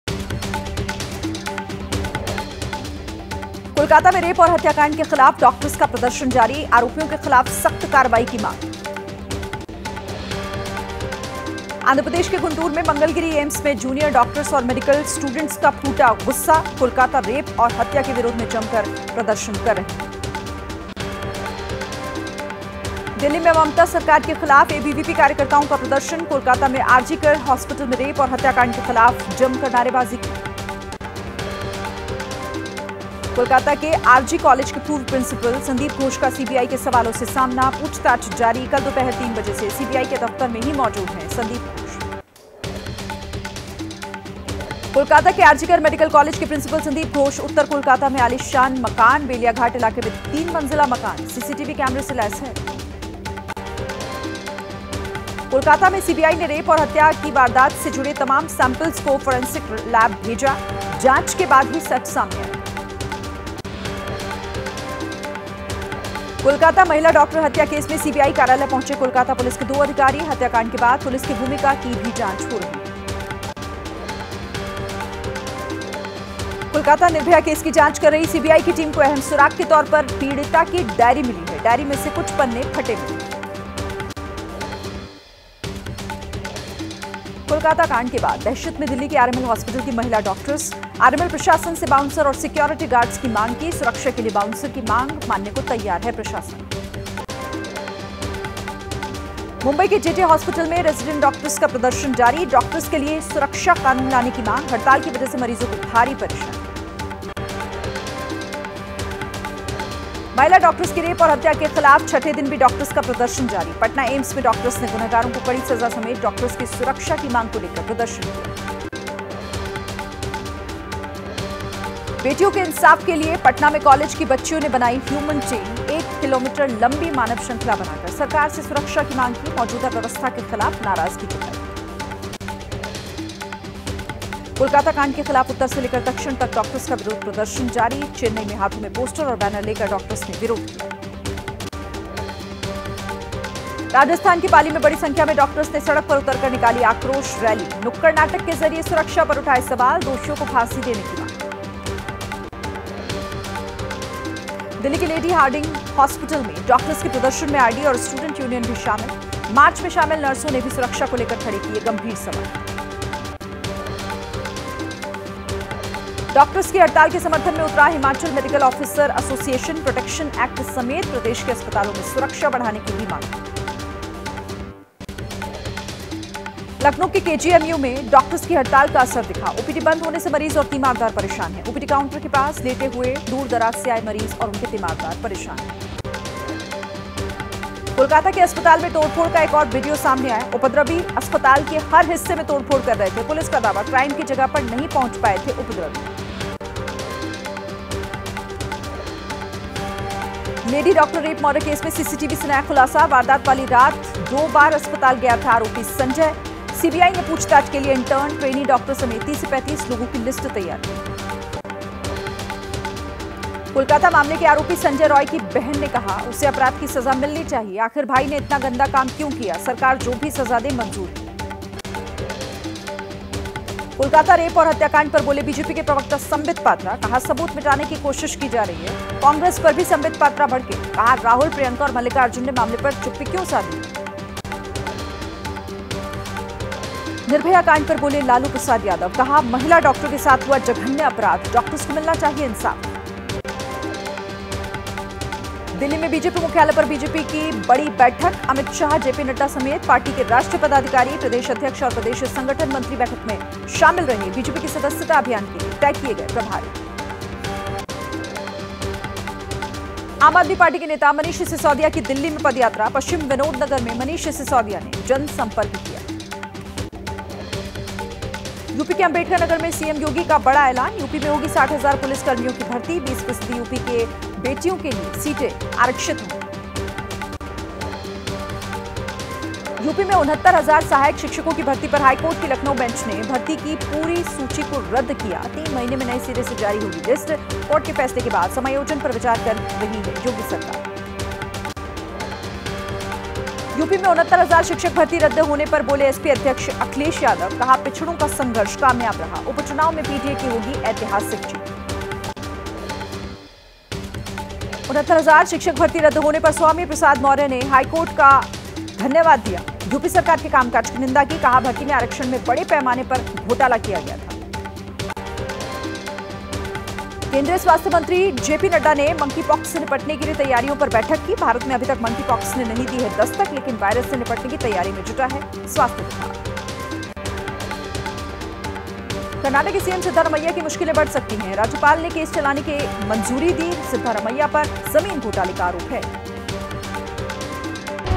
Top News: फटाफट अंदाज में देखिए बड़ी खबरें। Breaking | ABP News | Kolkata Doctor Case | BJP | Live
कोलकाता में रेप और हत्याकांड के खिलाफ डॉक्टरों का प्रदर्शन जारी. आरोपियों के खिलाफ सख्त कार्रवाई की मांग। आंध्रप्रदेश के गुंटूर में मंगलगिरी एम्स में जूनियर डॉक्टरों और मेडिकल छात्रों का फूटा गुस्सा...कोलकाता रेप और हत्या के विरोध में जमकर कर रहे प्रदर्शन। दिल्ली में ममता सरकार के खिलाफ एबीवीपी कार्यकर्ताओं का प्रदर्शन ...कोलकाता में आरजी अस्पताल में रेप और हत्याकांड के खिलाफ जमकर की नारेबाजी